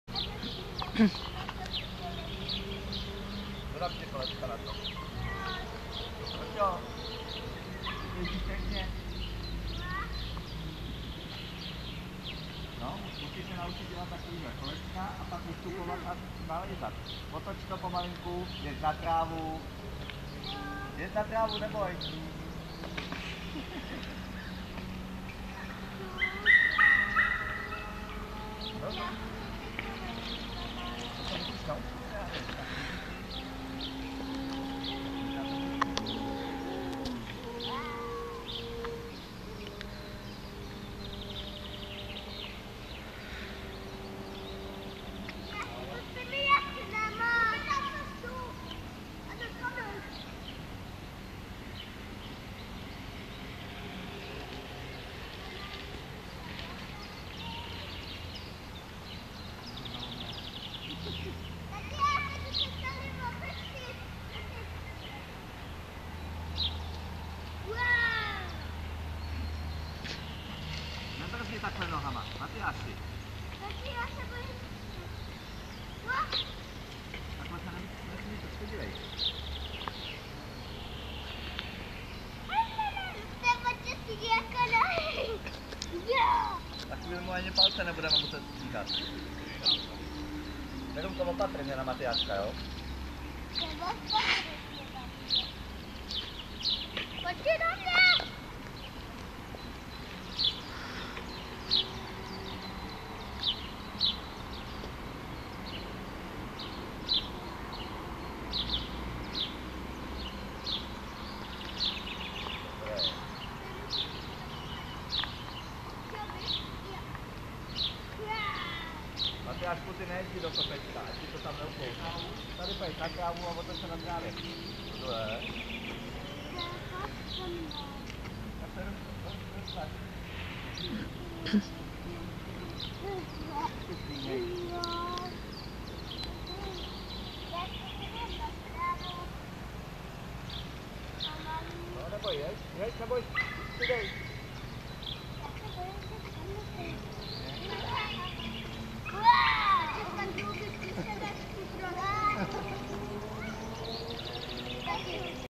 Matýsek
Poprvé bez postranních koleček.